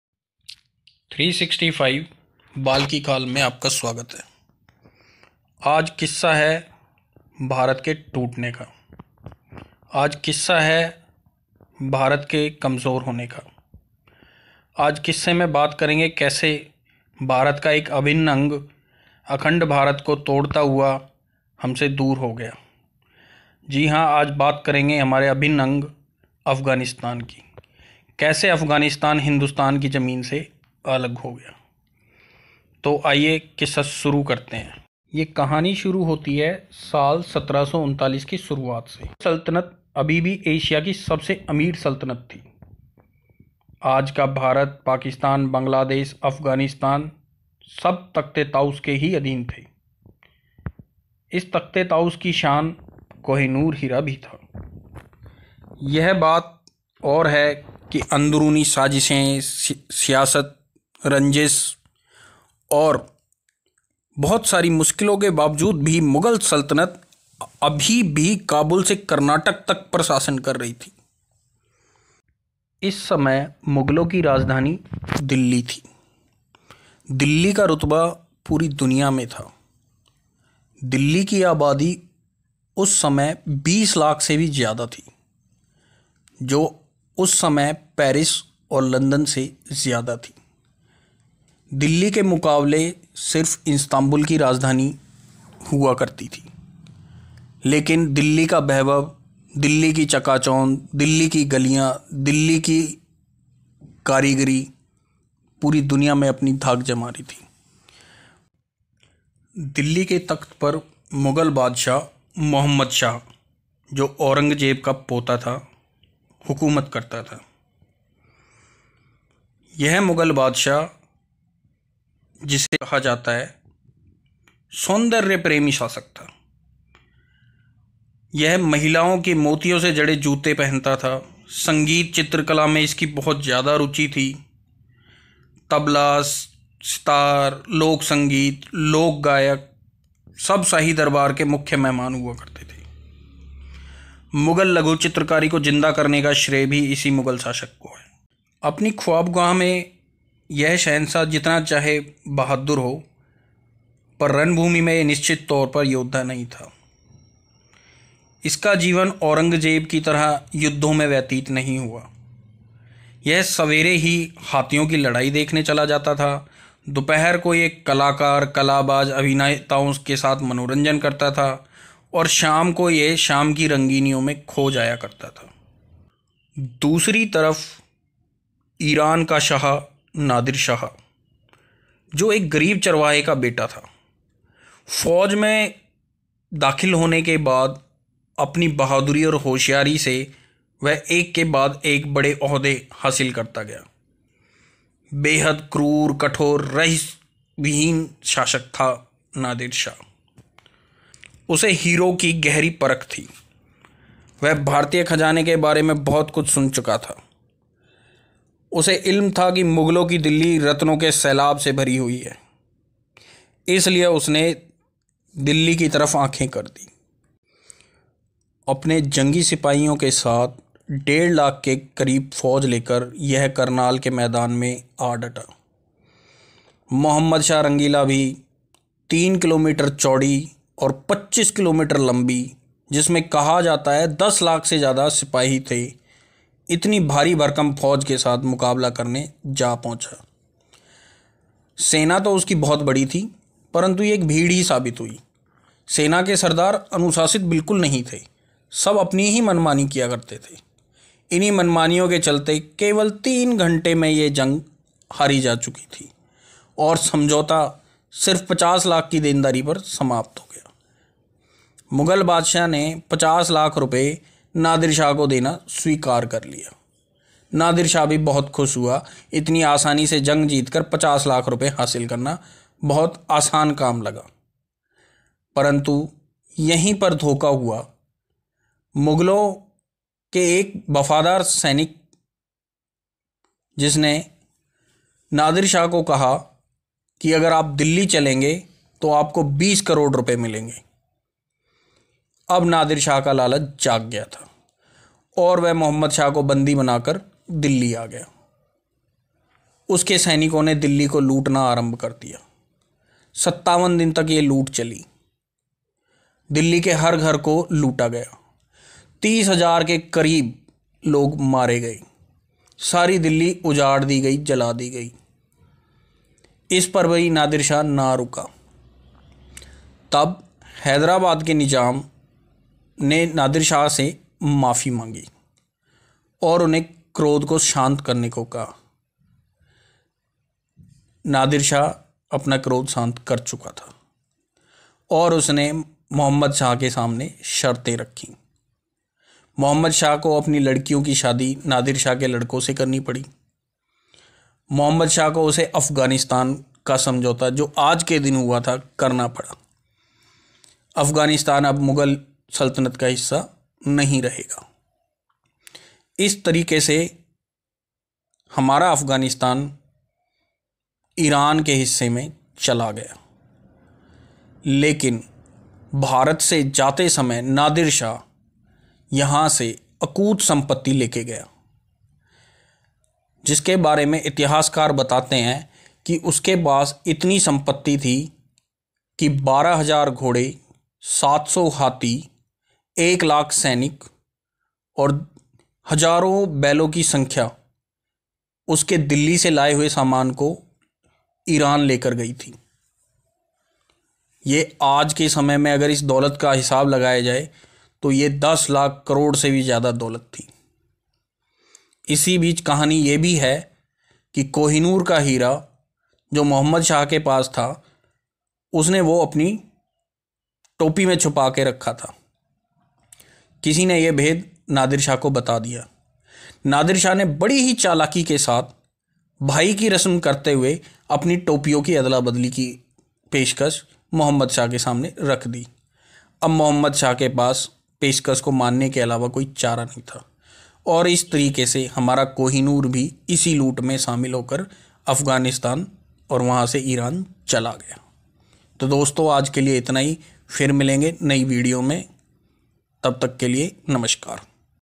History behind 26th May l Afganistan l Nadir Shah l Mohammad Shah Rangeela
This video is about the historic background of 26th May . kindly watch the video till end to know about the interesting facts about 26th may.